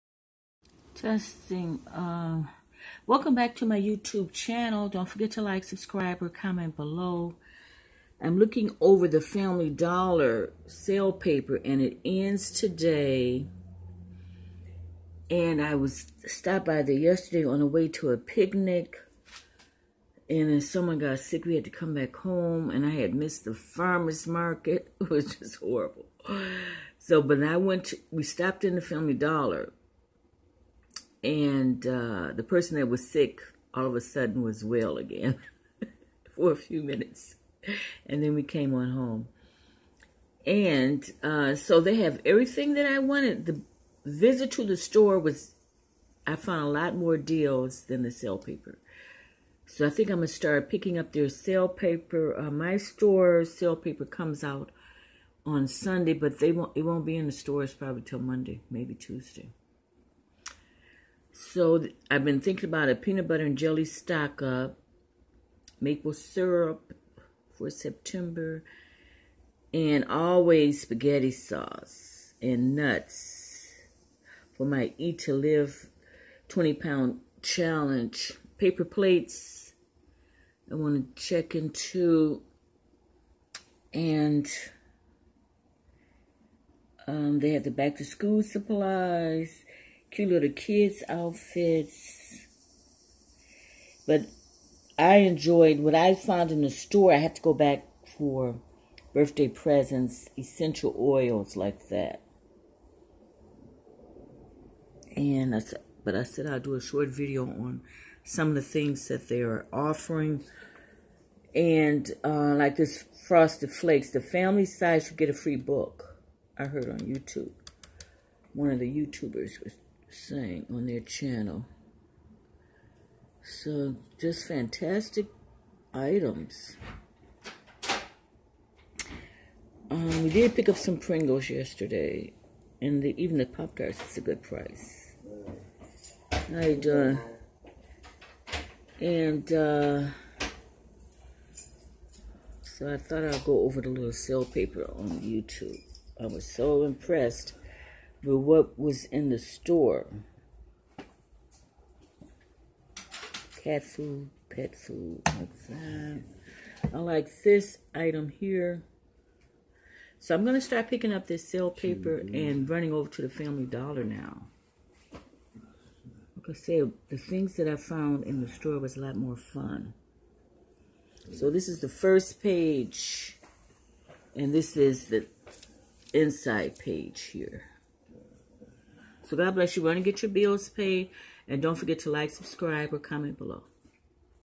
Family Dollar Review
Enjoy the rest of the summer and don't forget deals at the Family Dollar. I was extremely impressed. They have EVERYTHING! I wanted!